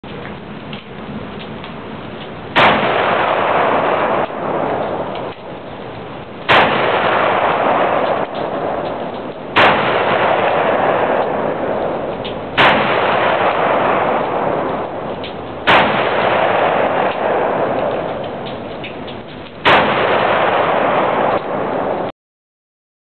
1847 Walker part 1
shooting 65 gr of FFFg behind a .457 round ball.  loading lever issue easily resolved with  a rubber band as can been seen in the second video